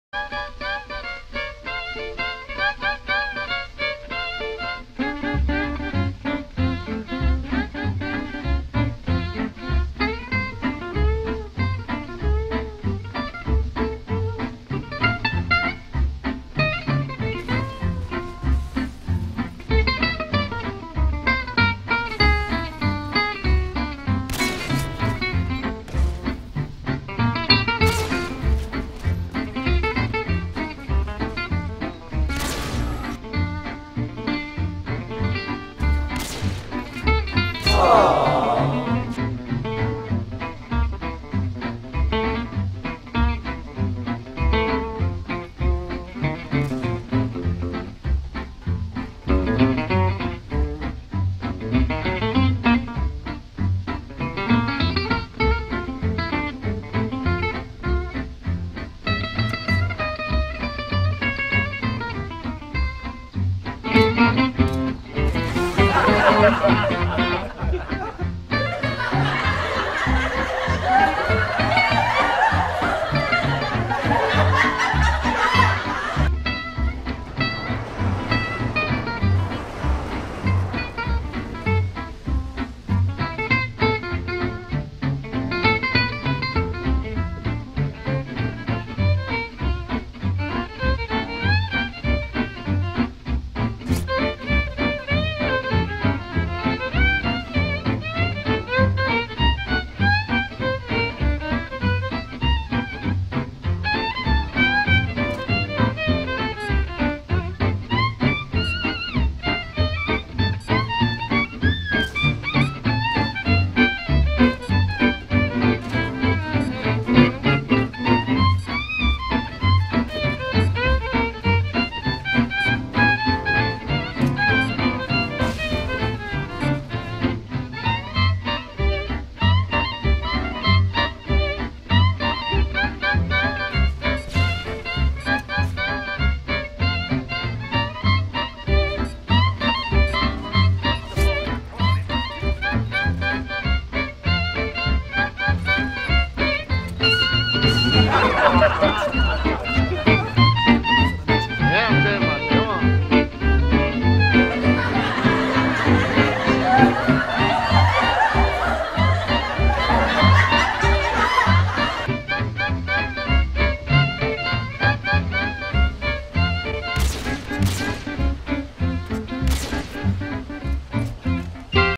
An Everyday Search And Destroy Round
Yep title is pretty self explaining. Had to laugh so hard about this round that you can't imagine trololol.
Players:
A4' N0bCaKD
Jizz Airwalkez
Sync Stewie
x  V I S